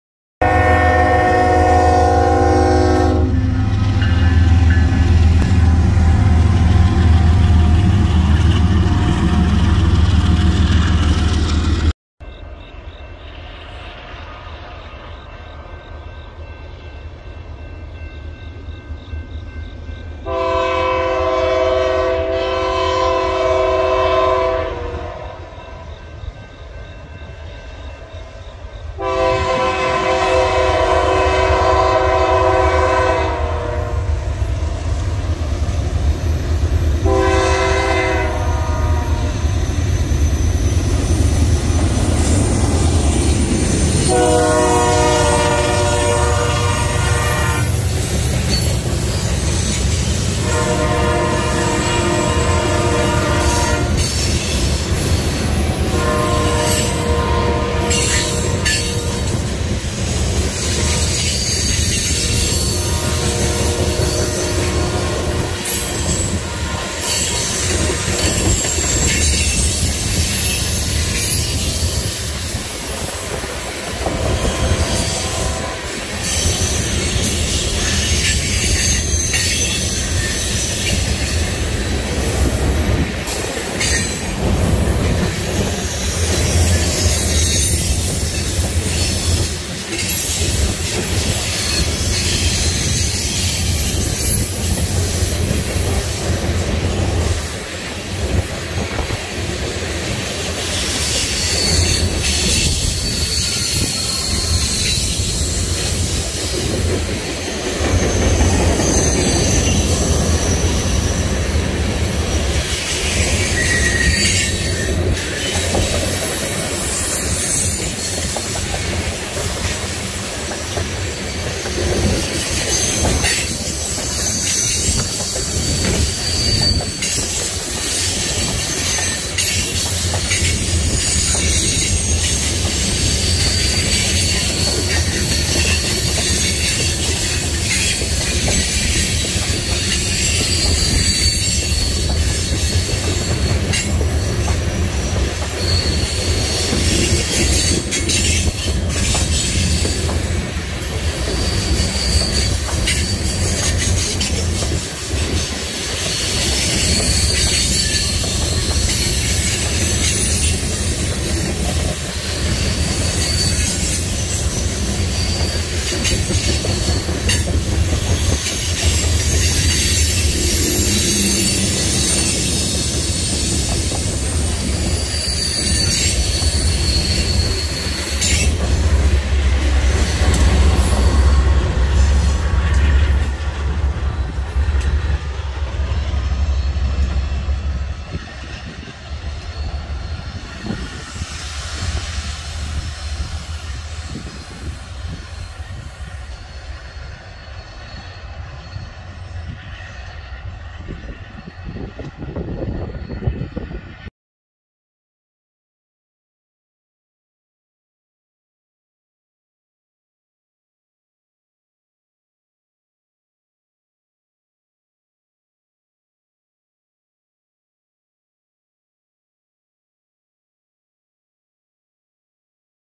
BNSF Grain Train with DPU Heading West Through Mendota, IL
Not too long after the Amtrak trains in my last video, this westbound BNSF grain train with DPU came through Mendota, IL.  It was really cool to see it bend around the curved tracks in Mendota.